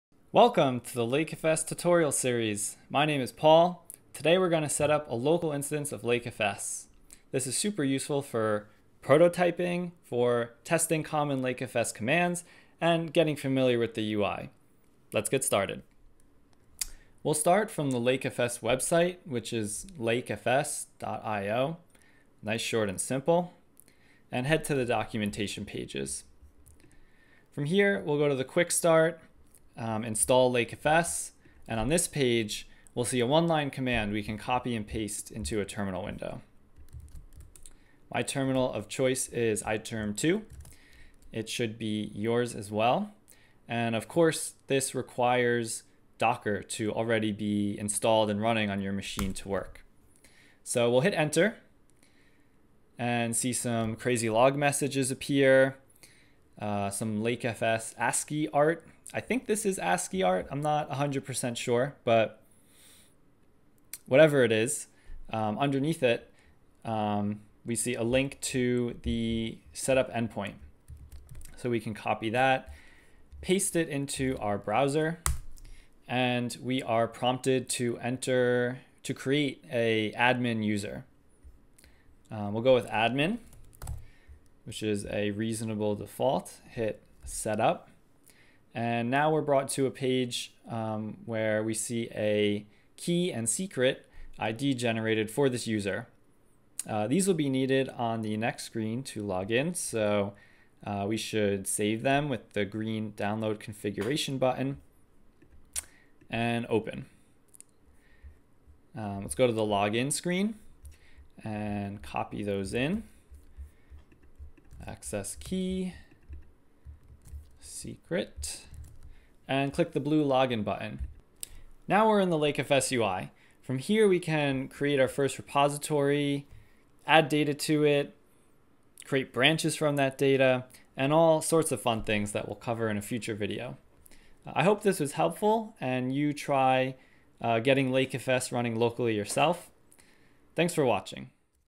lakeFS Tutorial Series: Install lakeFS Locally
In part of this tutorial series, we show how to get a local instance of lakeFS running on your laptop! We follow the instructions in the lakeFS Quickstart documentation